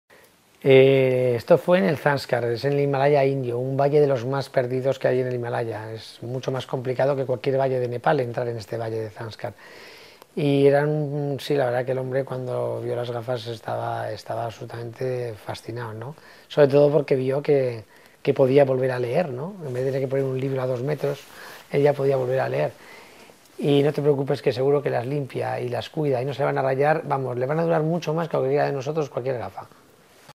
Pregunta Entrevista Jesús Calleja 28
Pregunta de Casa Rural Bigotes:
Me gusto mucho el detalle de las gafas que le llevaste al profesor de la escuela en un poblado creo que de Nepal, pero ¿le llevaste una gamuza para limpiarlas?. Mucho ánimo desde Arlanzón en la Sierra de la Demanda.